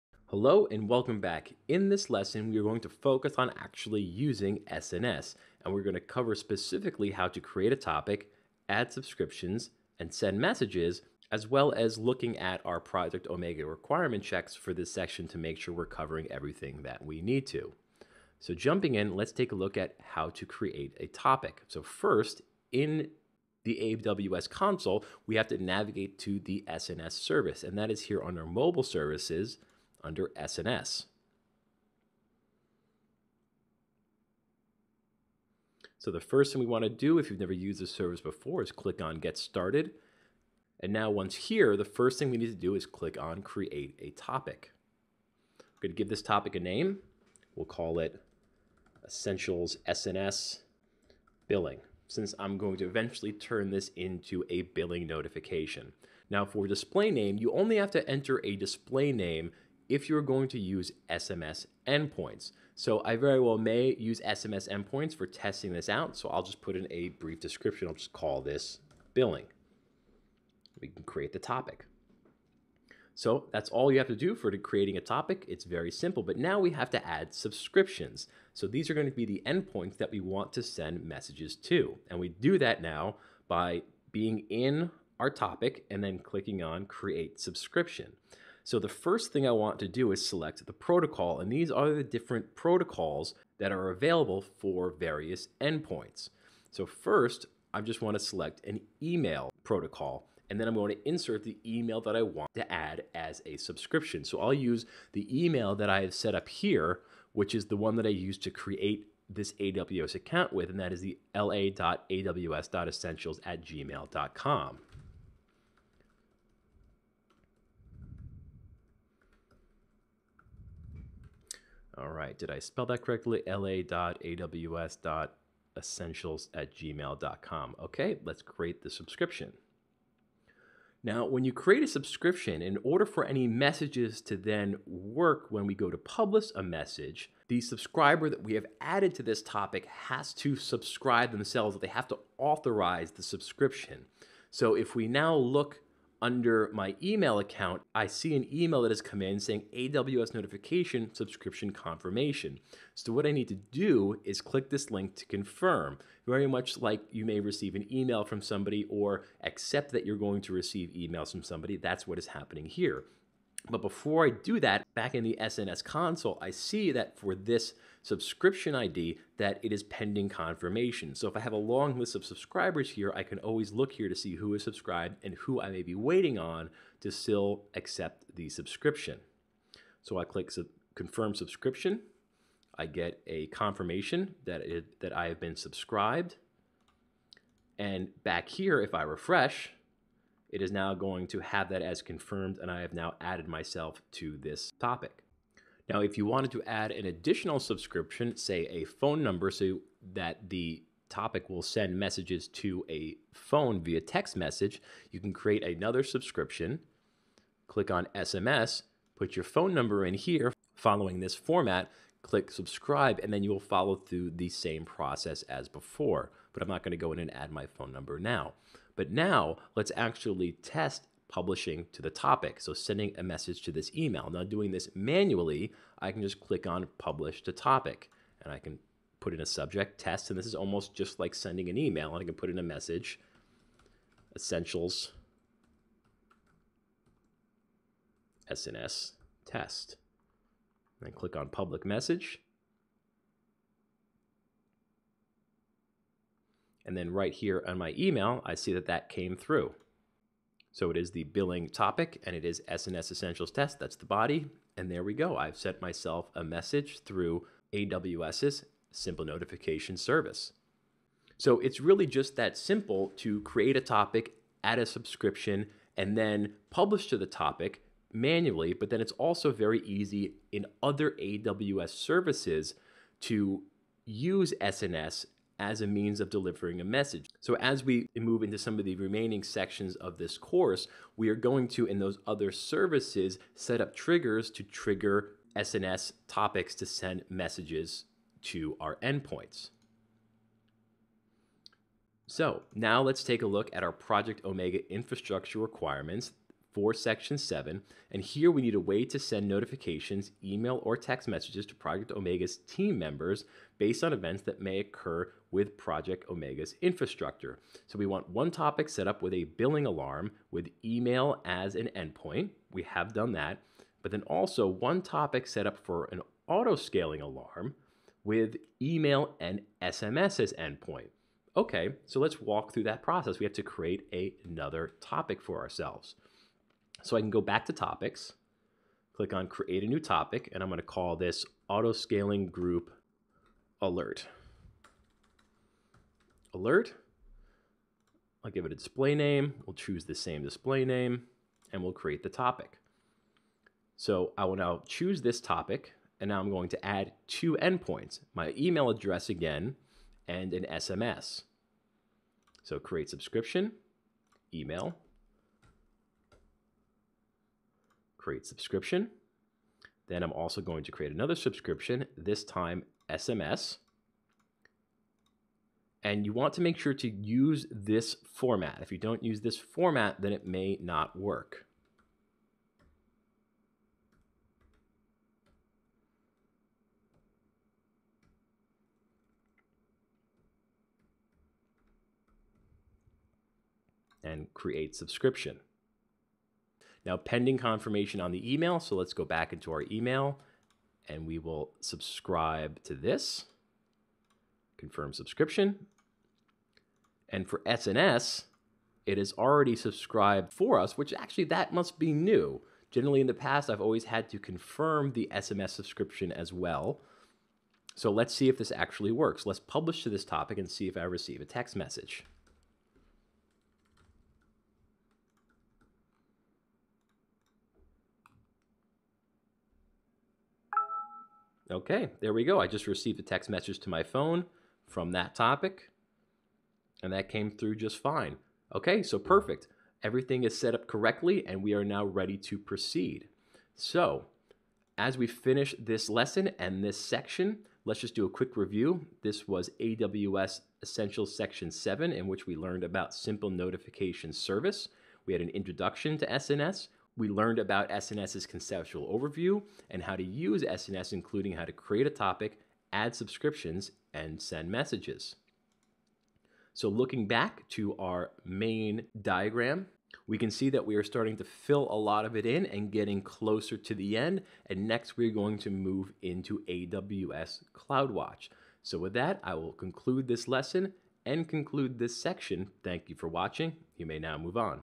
AWS Essentials: Using SNS
Let's walk through the processing of setting up the frame work for some SNS notifications.